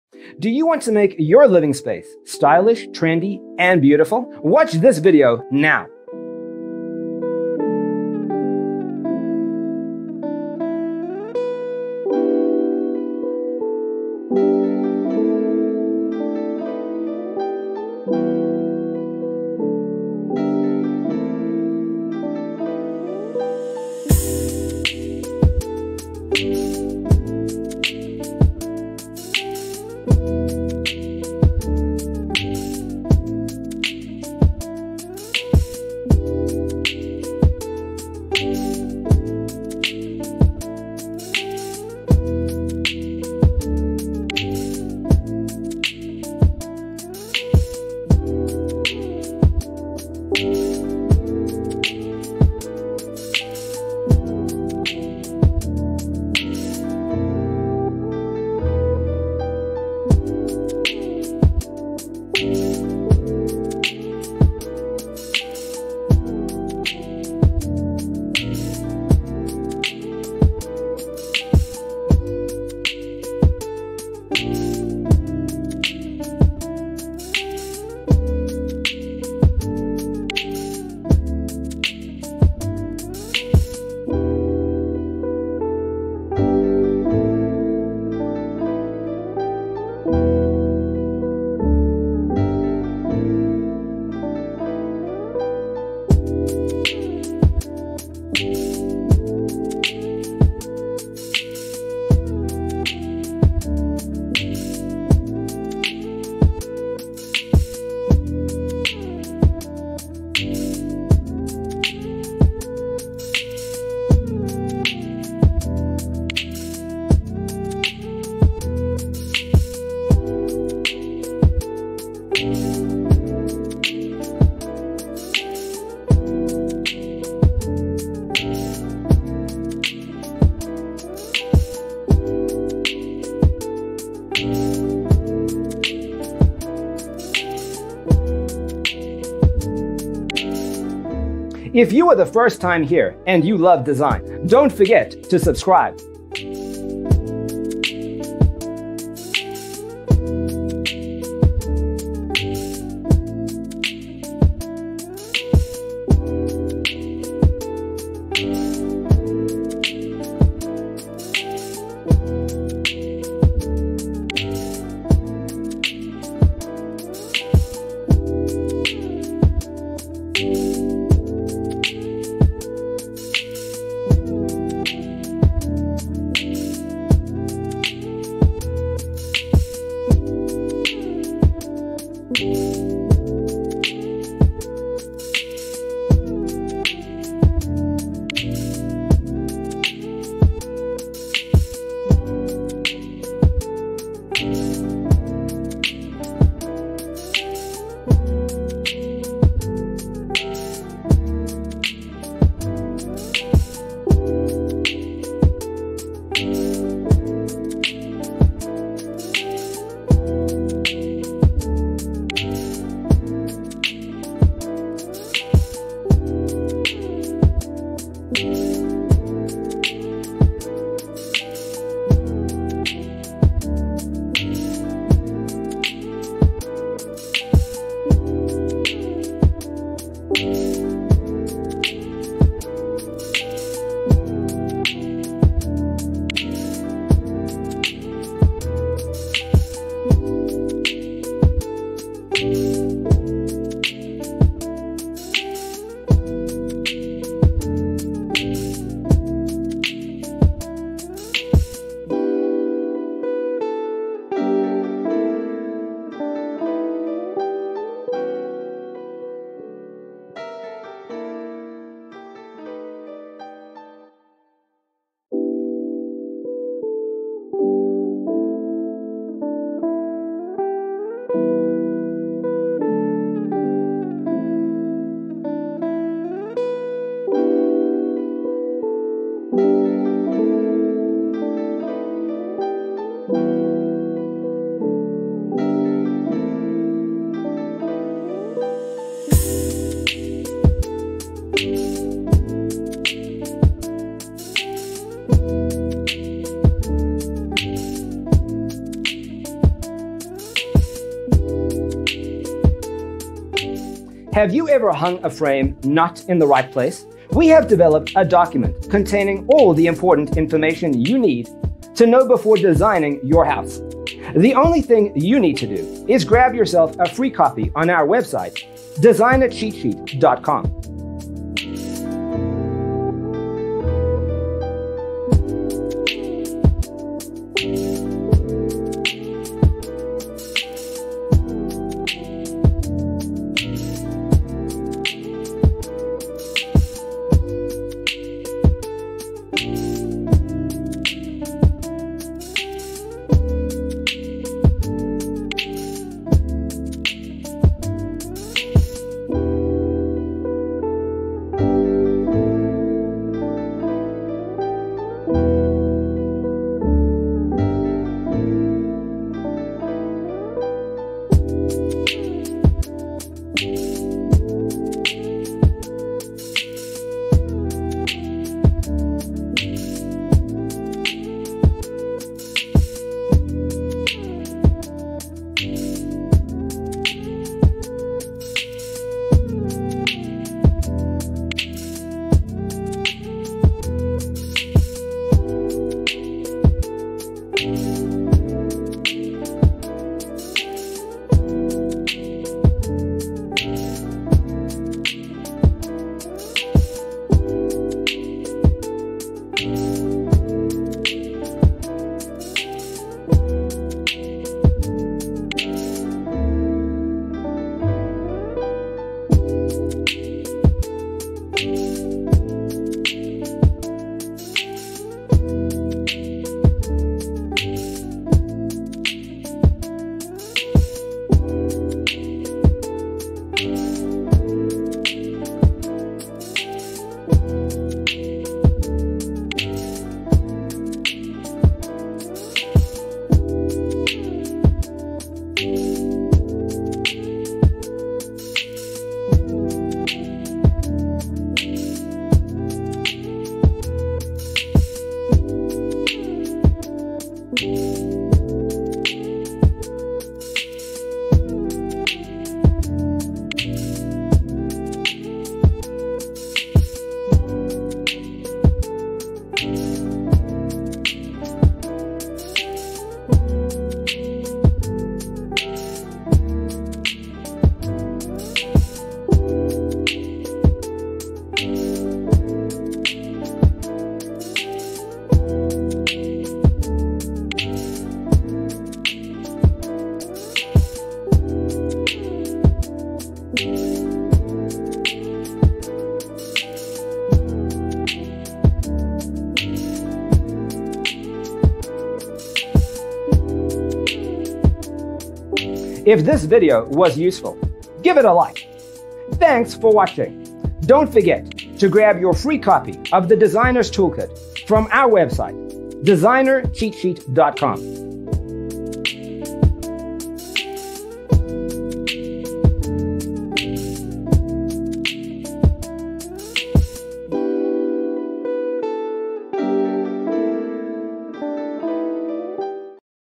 75 Gray Beige Floor Living Room Design Ideas You'll Love 🌈
Browse gray living room decorating ideas and furniture layouts. Discover design inspiration from a variety of gray living rooms, including color, decor and storage options.

0:00:00 - Intro
0:00:12 - Gray Beige Floor Living Room Design Ideas You'll Love: Inspirations 1 to 25
0:03:01 - Gray Beige Floor Living Room Design Ideas You'll Love: Inspirations 26 to 50
0:06:02 - Gray Beige Floor Living Room Design Ideas You'll Love: Inspirations 51 to 75

Hello there!🌈